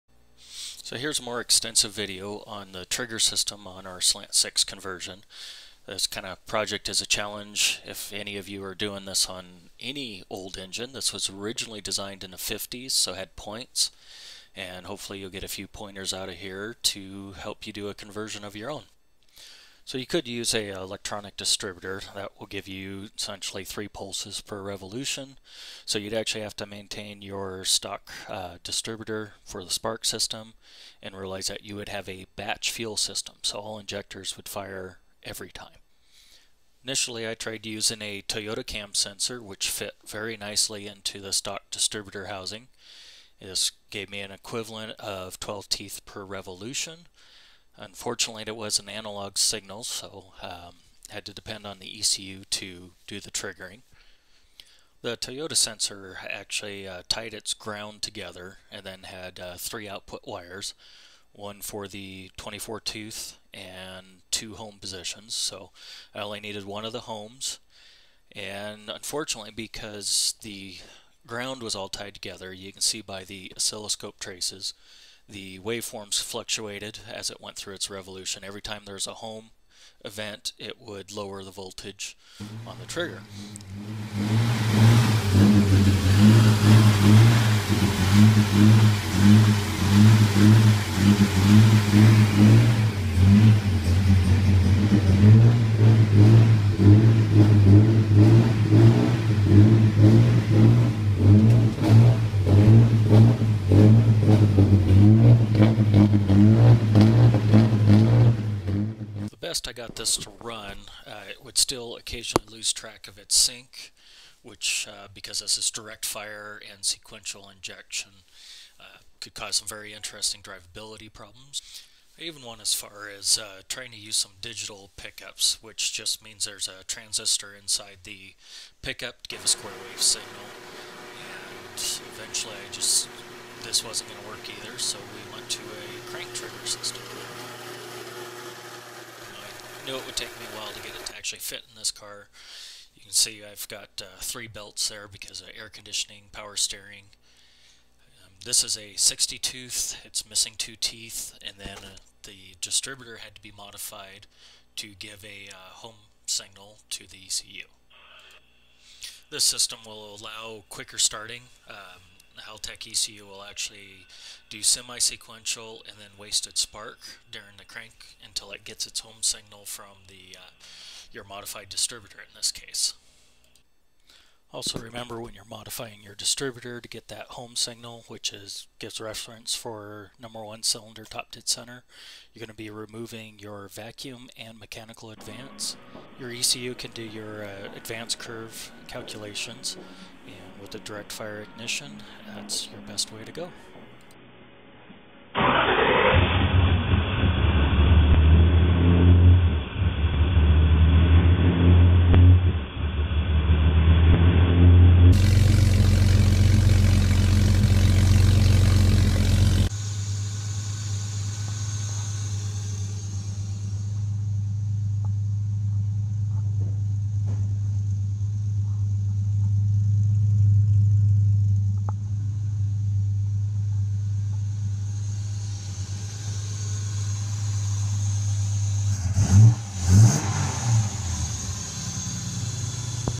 Slant 6 Duster EFI trigger
This video is about the trigger system on a Slant 6 that has been converted to EFI using a Haltech ECU.